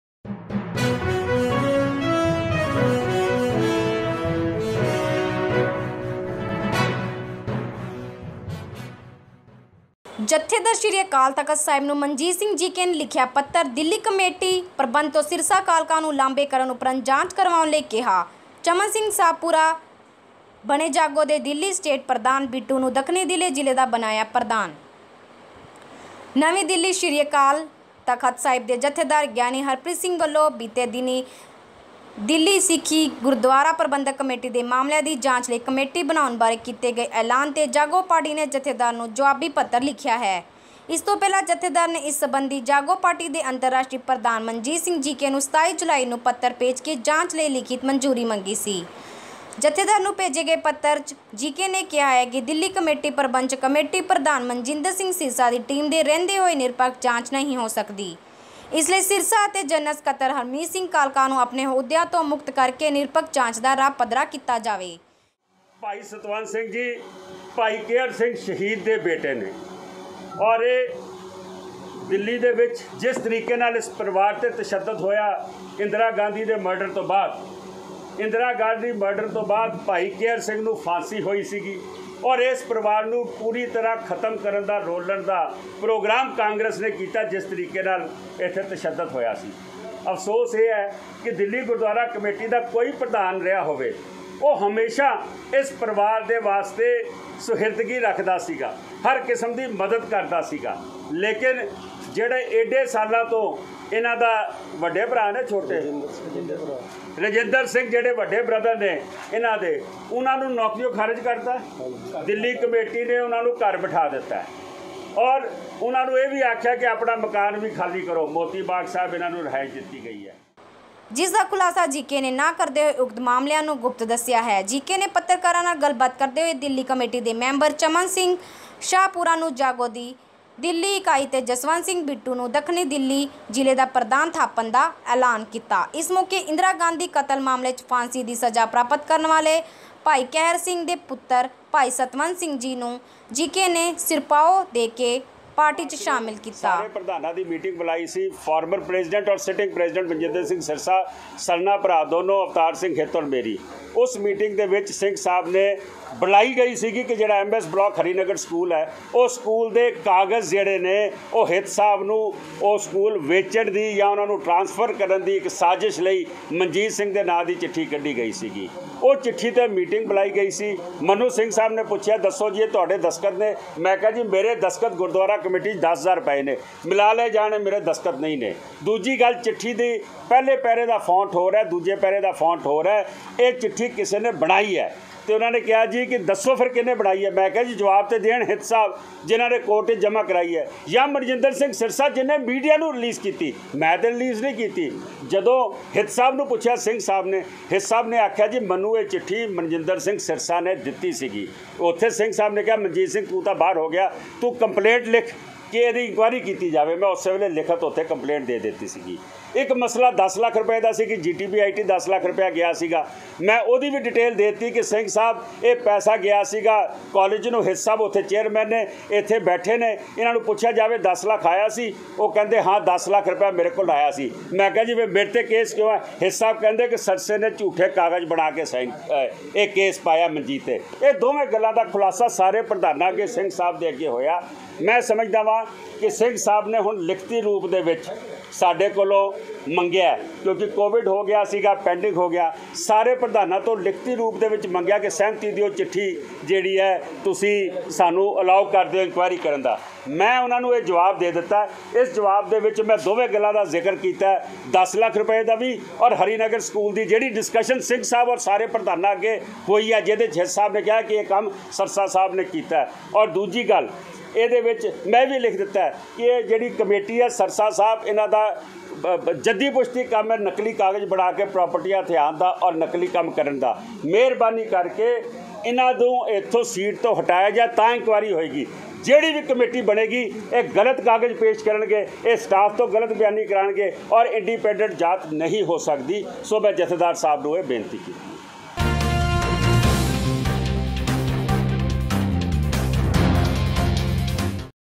*ਜਥੇਦਾਰ ਸ੍ਰੀ ਅਕਾਲ ਤਖਤ ਸਾਹਿਬ ਨੂੰ ਮਨਜੀਤ ਸਿੰਘ ਜੀਕੇ ਨੇ ਲਿਖਿਆ ਪੱਤਰ* *ਦਿੱਲੀ ਕਮੇਟੀ ਪ੍ਰਬੰਧ ਤੋਂ ਸਿਰਸਾ-ਕਾਲਕਾ
*ਜਥੇਦਾਰ ਸ੍ਰੀ ਅਕਾਲ ਤਖਤ ਸਾਹਿਬ ਨੂੰ ਮਨਜੀਤ ਸਿੰਘ ਜੀਕੇ ਨੇ ਲਿਖਿਆ ਪੱਤਰ*

*ਦਿੱਲੀ ਕਮੇਟੀ ਪ੍ਰਬੰਧ ਤੋਂ ਸਿਰਸਾ-ਕਾਲਕਾ ਨੂੰ ਲਾਂਭੇ ਕਰਨ ਉਪਰੰਤ ਜਾਂਚ ਕਰਵਾਉਣ ਲਈ ਕਿਹਾ*

*ਚਮਨ ਸਿੰਘ ਸਾਹਪੁਰਾ ਬਣੇ 'ਜਾਗੋ' ਦੇ ਦਿੱਲੀ ਸਟੇਟ ਪ੍ਰਧਾਨ*

*ਬਿੱਟੂ ਨੂੰ ਦੱਖਣੀ ਦਿੱਲੀ ਜਿਲ੍ਹੇ ਦਾ ਬਣਾਇਆ ਪ੍ਰਧਾਨ*

ਨਵੀਂ ਦਿੱਲੀ (12 ਅਗਸਤ 2020) ਸ੍ਰੀ ਅਕਾਲ ਤਖਤ ਸਾਹਿਬ ਦੇ ਜਥੇਦਾਰ ਗਿਆਨੀ ਹਰਪ੍ਰੀਤ ਸਿੰਘ ਵੱਲੋਂ ਬੀਤੇ ਦਿਨੀਂ ਦਿੱਲੀ ਸਿੱਖ ਗੁਰਦੁਆਰਾ ਪ੍ਰਬੰਧਕ ਕਮੇਟੀ ਦੇ ਮਾਮਲਿਆਂ ਦੀ ਜਾਂਚ ਲਈ ਕਮੇਟੀ ਬਣਾਉਣ ਬਾਰੇ ਕੀਤੇ ਗਏ ਐਲਾਨ 'ਤੇ 'ਜਾਗੋ' ਪਾਰਟੀ ਨੇ ਜਥੇਦਾਰ ਨੂੰ ਜੁਆਬੀ ਪੱਤਰ ਲਿਖਿਆ ਹੈ। ਇਸ ਤੋਂ ਪਹਿਲਾਂ ਜਥੇਦਾਰ ਨੇ ਇਸ ਸੰਬੰਧੀ 'ਜਾਗੋ' ਪਾਰਟੀ ਦੇ ਅੰਤਰਰਾਸ਼ਟਰੀ ਪ੍ਰਧਾਨ ਮਨਜੀਤ ਸਿੰਘ ਜੀਕੇ ਨੂੰ 27 ਜੁਲਾਈ ਨੂੰ ਪੱਤਰ ਭੇਜ ਕੇ ਜਾਂਚ ਲਈ ਲਿਖਿਤ ਮਨਜੂਰੀ ਮੰਗੀ ਸੀ। ਜਥੇਦਾਰ ਨੂੰ ਭੇਜੇ ਗਏ ਪੱਤਰ 'ਚ ਜੀਕੇ ਨੇ ਕਿਹਾ ਹੈ ਕਿ ਦਿੱਲੀ ਕਮੇਟੀ ਪ੍ਰਬੰਧ 'ਚ ਕਮੇਟੀ ਪ੍ਰਧਾਨ ਮਨਜਿੰਦਰ ਸਿੰਘ ਸਿਰਸਾ ਦੀ ਟੀਮ ਦੇ ਰਹਿੰਦੇ ਹੋਏ ਨਿਰਪੱਖ ਜਾਂਚ ਨਹੀਂ ਹੋ ਸਕਦੀ ਹੈਂ। ਇਸ ਲਈ ਸਿਰਸਾ ਅਤੇ ਜਨਰਲ ਸਕੱਤਰ ਹਰਮੀਤ ਸਿੰਘ ਕਾਲਕਾ ਨੂੰ ਆਪਣਿਆਂ ਅਹੁਦਿਆਂ ਤੋਂ ਮੁਕਤ ਕਰਕੇ ਹੀ ਨਿਰਪੱਖ ਜਾਂਚ ਦਾ ਰਾਹ ਪੱਧਰਾ ਕੀਤਾ ਜਾਵੇ। ਇਸਦੇ ਨਾਲ ਹੀ ਕਈ ਹੋਰ ਗੰਭੀਰ ਮਸਲੇ ਵੀ ਜੀਕੇ ਨੇ ਇਸ ਪੱਤਰ ਦੇ ਨਾਲ ਜਾਂਚ ਲਈ ਨੱਥੀ ਕਰਕੇ ਜਥੇਦਾਰ ਨੂੰ ਭੇਜੇ ਹਨ। ਜਿਸਦਾ ਖੁਲਾਸਾ ਜੀਕੇ ਨੇ ਨਾ ਕਰਦੇ ਹੋਏ ਉਕਤ ਮਾਮਲਿਆਂ ਨੂੰ ਗੁਪਤ ਦਸਿਆ ਹੈ। ਜੀਕੇ ਨੇ ਪਤਰਕਾਰਾਂ ਨਾਲ ਗਲਬਾਤ ਕਰਦੇ ਹੋਏ ਦਿੱਲੀ ਕਮੇਟੀ ਦੇ ਮੈਂਬਰ ਚਮਨ ਸਿੰਘ ਸ਼ਾਹਪੁਰਾ ਨੂੰ 'ਜਾਗੋ' ਦੀ ਦਿੱਲੀ ਇਕਾਈ ਅਤੇ ਜਸਵੰਤ ਸਿੰਘ ਬਿੱਟੂ ਨੂੰ ਦਖਣੀ ਦਿੱਲੀ ਜਿਲ੍ਹੇ ਦਾ ਪ੍ਰਧਾਨ ਥਾਪਣ ਦਾ ਐਲਾਨ ਕੀਤਾ। ਇਸ ਮੌਕੇ ਇੰਦਰਾ ਗਾਂਧੀ ਕਤਲ ਮਾਮਲੇ 'ਚ ਫਾਂਸੀ ਦੀ ਸਜਾ ਪ੍ਰਾਪਤ ਕਰਨ ਵਾਲੇ ਭਾਈ ਕੇਹਰ ਸਿੰਘ ਦੇ ਪੁੱਤਰ ਭਾਈ ਸਤਵੰਤ ਸਿੰਘ ਨੂੰ ਜੀਕੇ ਨੇ ਸਿਰੋਪਾਓ ਦੇਕੇ ਪਾਰਟੀ 'ਚ ਸ਼ਾਮਿਲ ਕੀਤਾ।

ਜੀਕੇ ਨੇ ਪੱਤਰ 'ਚ ਜਥੇਦਾਰ ਵੱਲੋਂ 5 ਮਾਰਚ 2020 ਨੂੰ ਸ੍ਰੀ ਅਕਾਲ ਤਖਤ ਸਾਹਿਬ ਸਕੱਤਰੇਤ ਵਿਖੇ ਦਿੱਲੀ ਕਮੇਟੀ ਦੇ ਸਾਬਕਾ ਪ੍ਰਧਾਨ ਸਾਹਿਬਾਨ ਪਰਮਜੀਤ ਸਿੰਘ ਸਰਨਾ, ਹਰਵਿੰਦਰ ਸਿੰਘ ਸਰਨਾ ਤੇ ਅਵਤਾਰ ਸਿੰਘ ਹਿਤ, ਮੌਜੂਦਾ ਪ੍ਰਬੰਧਕ ਸਿਰਸਾ ਤੇ ਕਾਲਕਾ ਅਤੇ ਖੁਦ ਦੀ ਹੋਈ ਪੇਸ਼ੀ 'ਚ ਅਗਲੇਰੀ ਜਾਂਚ ਲਈ ਵਿਚਾਰੇ ਗਏ ਮਾਮਲਿਆਂ ਬਾਰੇ ਜਥੇਦਾਰ ਵੱਲੋਂ ਮੰਗੀ ਗਈ ਲਿਖਿਤ ਪ੍ਰਵਾਨਗੀ ਬਾਰੇ ਕਿਹਾ ਹੈ ਕਿ ਮੇਰੇ ਵੱਲੋਂ ਜਾਂਚ ਨੂੰ ਪਹਿਲਾਂ ਹੀ ਪ੍ਰਵਾਨਗੀ ਸੀ ਤੇ ਹੁਣ ਵੀਂ ਹੈ। ਪਰ ਫਿਰ ਵੀ ਮੈਂ ਹੁਣ ਲਿਖਿਤ ਪ੍ਰਵਾਨਗੀ ਭੇਜ ਰਿਹਾ ਹੈ। ਇਸ ਮੀਟਿੰਗ ਦੌਰਾਨ ਆਪ ਜੀ ਦੀ ਮੌਜੂਦਗੀ 'ਚ ਕੁਝ ਵਿਚਾਰਾਂ ਹੋਇਆ ਸਨ, ਜਿਸ ਵਿੱਚ ਗੁਰੂ ਹਰਿਕ੍ਰਿਸ਼ਨ ਪਬਲਿਕ ਸਕੂਲ, ਹਰੀ ਨਗਰ ਦਾ ਮਾਮਲਾ ਵੀਂ ਆਇਆ ਸੀ। ਆਪ ਜੀ ਨੇ ਇਸ ਮਾਮਲੇ ਵਿੱਚ ਮੇਰੇ ਨਾਂਅ 'ਤੇ ਹਿੱਤ ਨੂੰ ਸਕੂਲ ਦੇਣ ਦੀ ਚਲ ਰਹੀ ਕਥਿਤ ਚਿੱਠੀ ਬਾਰੇ ਮੇਰੇ ਤੋਂ ਪੁਛਿਆ ਸੀ। ਜਿਸ ਬਾਰੇ ਮੇਰਾ ਕਹਿਣਾ ਸੀ ਕੀ "ਨਾ ਤਾਂ ਮੈਂ ਇਹ ਚਿੱਠੀ ਕਿਸੇ ਨੂੰ ਦਿੱਤੀ ਹੈਂ ਅਤੇ ਨਾ ਹੀ ਇਸ 'ਤੇ ਮੇਰੇ ਦਸਤਖਤ ਹਨ। ਨਾਲ ਹੀ ਚਿੱਠੀ ਵਿਚ ਪਹਿਲੇ ਪਹਿਰੇ ਦਾ ਫੌਂਟ ਸਾਈਜ ਅਤੇ ਸਟਾਇਲ, ਦੂਜੇ ਪਹਿਰੇ ਨਾਲੋਂ ਵੱਖਰਾ ਹੈਂ। ਇਸ ਕਰਕੇ ਚਿਠੀ ਫਰਜ਼ੀ ਹੈਂ। ਉਕਤ ਚਿੱਠੀ ਨੂੰ ਹਿਤ ਨੇ ਕੋਰਟ ਵਿੱਚ ਜਮਾ ਕਰਵਾਇਆ ਸੀ ਤੇ ਸਿਰਸਾ ਨੇ ਦਿੱਲੀ ਕਮੇਟੀ ਦੀ ਅਧਿਕਾਰਤ ਈ-ਮੇਲ ਰਾਹੀ ਮੀਡੀਆ ਨੂੰ ਜਾਰੀ ਕੀਤਾ ਸੀ। ਇਸ ਕਰਕੇ ਹਿਤ ਨੂੰ ਪੁਛਿਆ ਜਾਵੇ ਕੀ ਉਕਤ ਚਿੱਠੀ ਉਹਨਾਂ ਨੂੰ ਕਿਵੇਂ ਮਿੱਲੀ ?" ਤਦ ਹਿਤ ਨੇ ਕਿਹਾ ਸੀ ਕੀ ਉਕਤ ਚਿੱਠੀ ਸਿਰਸਾ ਨੇ ਉਹਨਾਂ ਨੂੰ ਕੋਰਟ 'ਚ ਜਮ੍ਹਾਂ ਕਰਵਾਉਣ ਵਾਸਤੇ ਦਿੱਤੀ ਸੀ। ਜਿਸ ਦੇ ਬਾਅਦ ਆਪ ਜੀ ਨੇ ਕਿਹਾ ਸੀ ਕੀ ਮੈਨੂੰ ਸਾਰੀ ਗੱਲ ਸਮਞ ਆ ਗਈ ਹੈਂ, ਆਪ ਲਿਖਤੀ ਰੂਪ 'ਚ ਸ੍ਰੀ ਅਕਾਲ ਤਖਤ ਸਾਹਿਬ ਨੂੰ ਪੱਤਰ ਦੇ ਕੇ ਇਸ ਦੀ ਜਾਂਚ ਦੀ ਮੰਗ ਕਰੋ। ਜਿਸ ਉਪਰੰਤ ਮੈਂ ਆਪ ਨੂੰ ਲਿਖਤੀ ਬੇਨਤੀ ਪਤੱਰ ਵੀ ਦਿੱਤਾ ਸੀ।

ਦੂਜਾ ਮਾਮਲਾ ਦਾਸ ਨੇ ਆਪ ਜੀ ਦੇ ਨੋਟਿਸ ਵਿੱਚ ਲਿਆਂਦਾ ਸੀ, ਜਿਹੜਾ ਕਮੇਟੀ ਦੇ ਤਕਨੀਕੀ ਅਦਾਰੇ ਗੁਰੂ ਤੇਗ ਬਹਾਦਰ ਇੰਸਟੀਚਿਊਟ ਆਫ ਟੈਕਨਾਲੋਜੀ (GTBIT) ਨੂੰ 10 ਲੱਖ ਰੁਪਏ ਦੀ ਰਕਮ ਕਮੇਟੀ ਵੱਲੋਂ ਦਿੱਤੇ ਜਾਣ ਨਾਲ ਸੰਬੰਧਿਤ ਸੀ। ਜਿਸ ਬਾਰੇ ਕਮੇਟੀ ਨੇ ਮੇਰੇ ਖਿਲਾਫ ਰੁਪਏ ਦੇ ਗਬਨ ਦਾ ਝੂਠਾ ਮਾਮਲਾ ਪੁਲਿਸ ਨੂੰ ਦਿੱਤਾ ਹੋਇਆ ਹੈਂ। ਉਸ ਬਾਰੇ ਜਦੋਂ ਦਾਸ ਨੇ ਸਾਰਿਆਂ ਦੀ ਹਾਜਰੀ ਵਿੱਚ ਹਿਤ ਨੂੰ (ਜੋ ਕਿ ਉਸ ਅਦਾਰੇ ਦੇ ਚੇਅਰਮੈਨ ਹਨ) ਉਕਤ ਰਕਮ ਦੇ ਅਦਾਰੇ ਵਿੱਚ ਆਉਣ ਬਾਰੇ ਸਵਾਲ ਕੀਤਾ ਕੀ ਇਹ 10 ਲੱਖ ਰੁਪਏ ਅਦਾਰੇ ਵਿੱਚ ਆਏ ਸਨ ? ਤਾਂ ਹਿਤ ਨੇ ਉਕਤ ਰਕਮ ਵਸੂਲ ਪਾਏ ਜਾਣ ਦੀ ਸਾਰਿਆਂ ਸਾਹਮਣੇ ਹਾਮੀ ਭਰੀ ਸੀ। ਉਕਤ ਦੋ ਹਵਾਲਿਆਂ ਤੋਂ ਸਪਸ਼ਟ ਹੈਂ ਕਿ ਦਾਸ ਨੂੰ ਗਲਤ ਅਤੇ ਚੋਰ ਸਾਬਤ ਕਰਨ ਲਈ ਸਿਰਸਾ ਦੀ ਟੀਮ ਬੜੀ ਚਲਾਕੀ ਨਾਲ ਨਕਲੀ ਕਾਗਜ਼ ਬਣਾਉਣ ਦਾ ਕਾਰਜ ਕਰ ਰਹੀ ਹੈ। ਇਸ ਦੇ ਨਾਲ ਹੀ ਮੇਰੇ ਕੋਲ ਜਾਣਕਾਰੀ ਹੈਂ ਕਿ ਮੇਰੇ ਖਿਲਾਫ ਝੂਠੀਆਂ ਗਵਾਹੀਆਂ ਦੇਣ ਲਈ ਸਟਾਫ ਨੂੰ ਮਜਬੂਰ ਕੀਤਾ ਜਾ ਰਿਹਾ ਹੈਂ। ਸਟਾਫ ਨੂੰ ਨੌਕਰੀ ਤੋਂ ਕੱਢਣ ਤੇ ਰਿਹਾਇਸ਼ ਕਮੇਟੀ ਵੱਲੋਂ ਖਾਲੀ ਕਰਵਾਉਣ ਦੀ ਧਮਕੀਆਂ ਵੀ ਦਿੱਤੀਆਂ ਜਾ ਹਰਿਆ ਹਨ। ਸਿਰਸਾ ਨੂੰ ਝੂਠੇ ਕਾਗਜ਼ਤ ਤਿਆਰ ਕਰਨ 'ਚ ਮੁਹਾਰਤ ਹਾਸਲ ਹੈ ਅਤੇ ਬਹੁਤ ਸਾਰੇ ਅਜਿਹੇ ਕੰਮ ਮੈਨੂੰ ਫਸਾਉਣ ਅਤੇ ਮੇਰੀ ਛਵੀ ਨੂੰ ਖਰਾਬ ਕਰਨ ਵਾਸਤੇ ਇਹਨਾਂ ਕੀਤੇ ਵੀ ਹਨ।

Karanjeet singh...